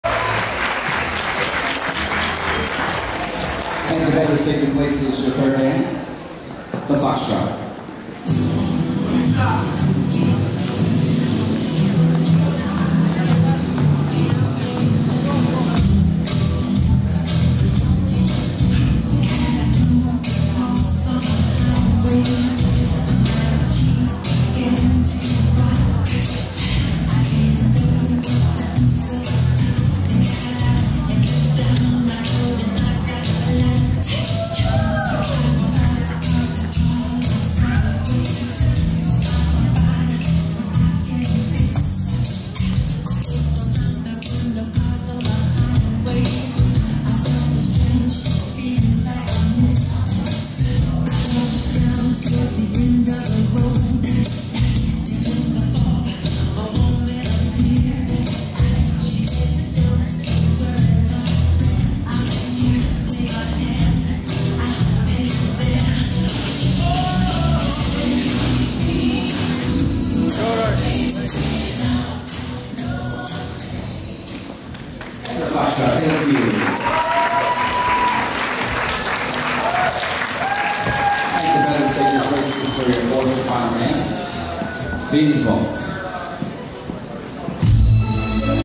Pauline Adams and Matt Rivers: Am. Smooth Foxtrot, Chicago Harvest Moon
American Smooth Foxtrot, Chicago Harvest Moon Ball, October 14, 2023